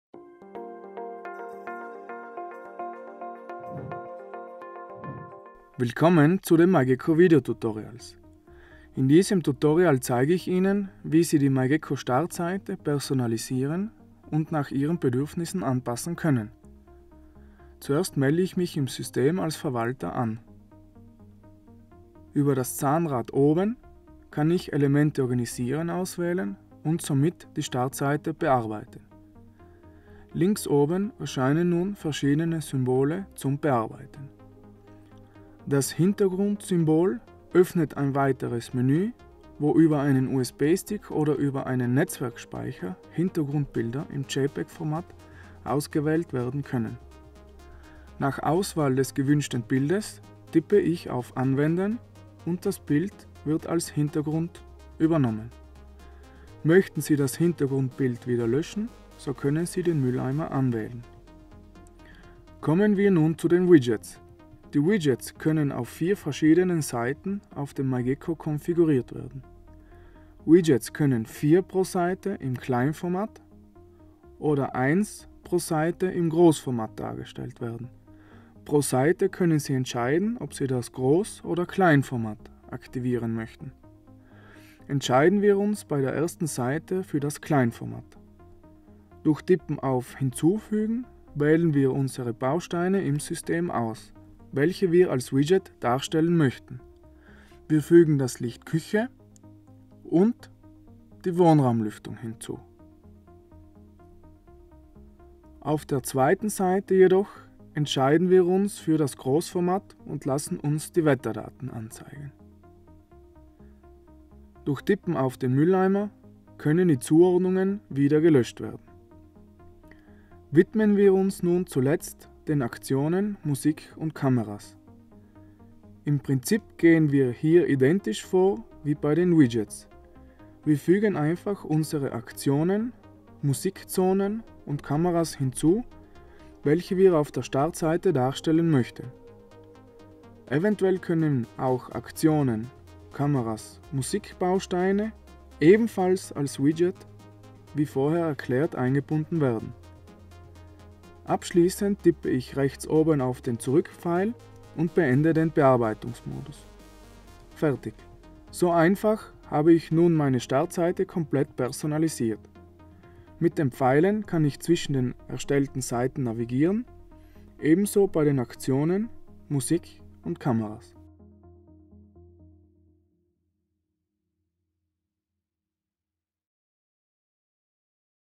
Personalisieren der Startseite │ myGEKKO Tutorial
Wie personalisiere ich meine myGEKKO Startseite, damit ich auf den ersten Blick meine wichtigen Informationen und Schnelltasten finde?
In diesem Videotutorial zeige ich Ihnen, wie einfach Sie myGEKKO personalisieren können.

Anmelden als Verwalter - 00:17
Auswählen des Hintergrundes - 00:36
Auswählen der Widgets - 01:02
Auswählen Aktionen/Musik/Kameras - 01:58